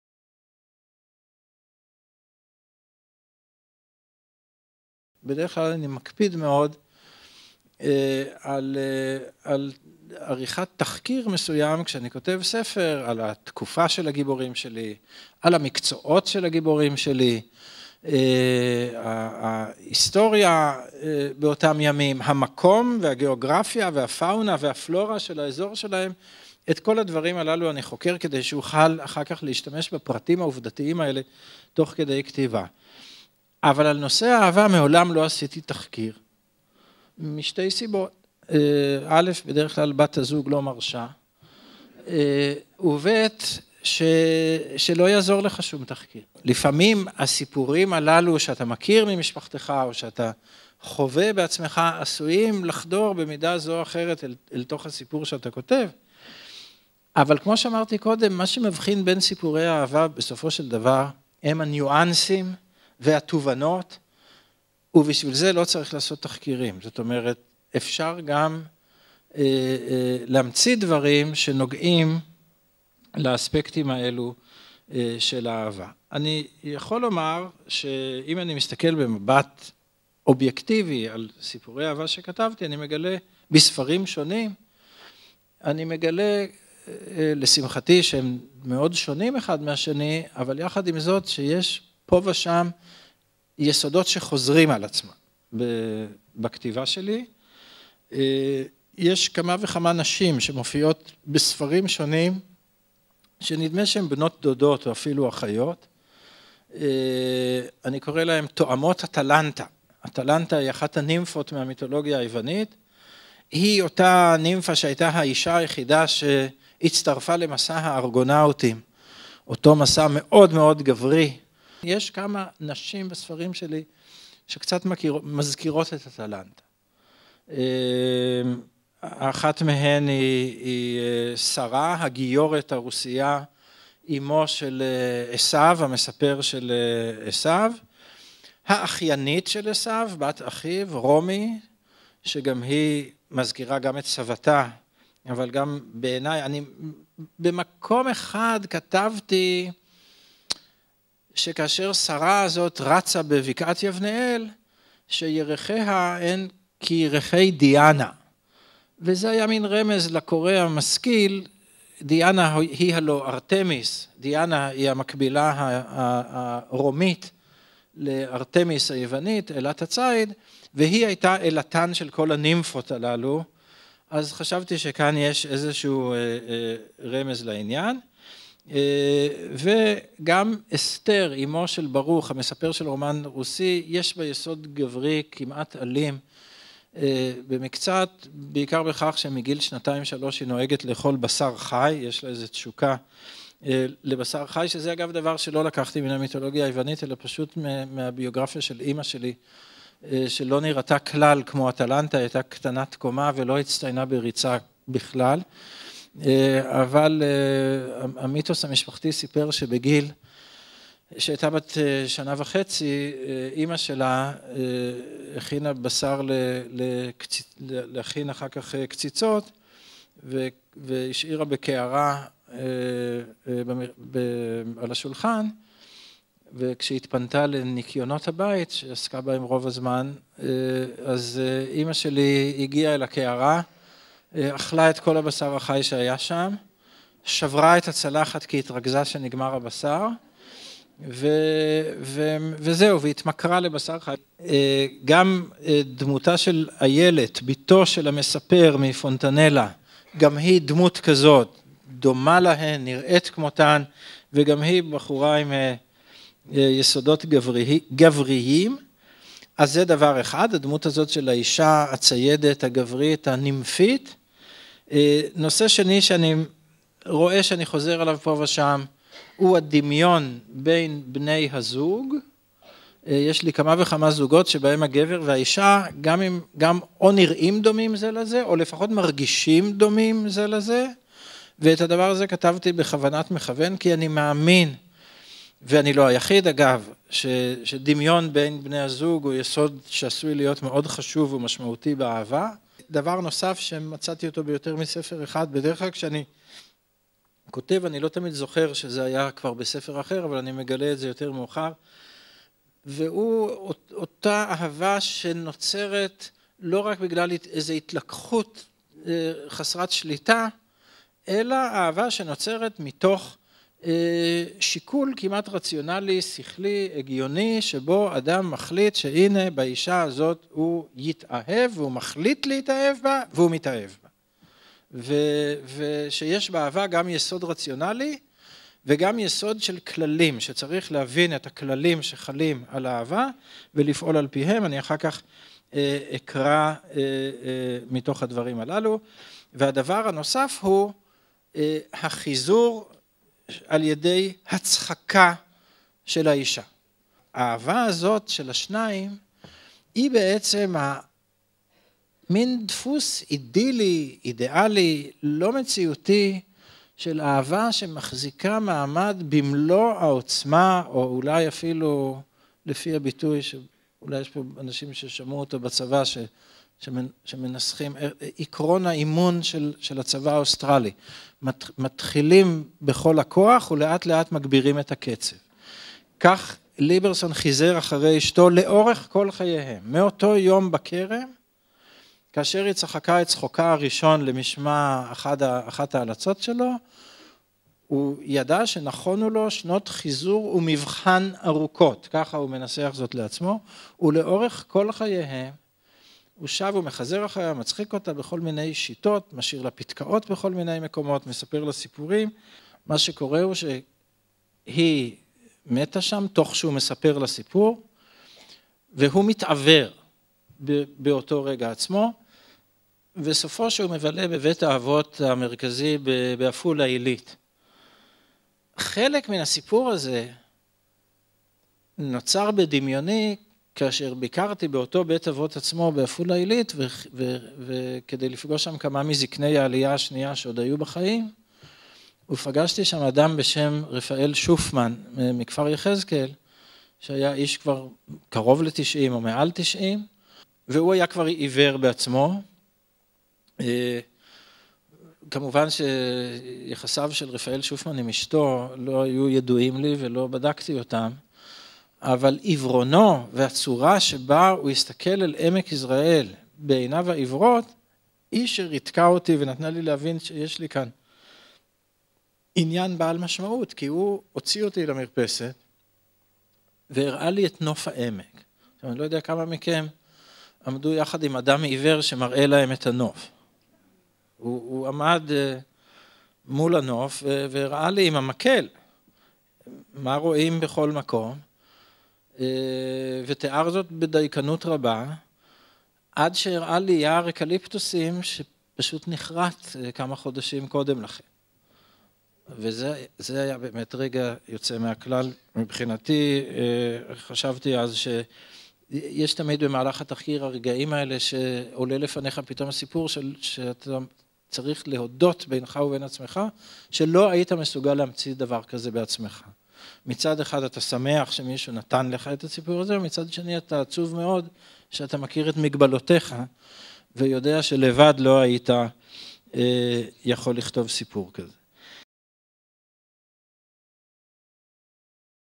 מתוך הרצאתו של הסופר מאיר שלו - סיפורי אהבה בספרים: עשו, פונטנלה ורומן רוסי
הסופר מאיר שלו מתוך הרצאתו בקתדרה - סיפורי אהבה בספרים עשו, פונטנלה ורומן רוסי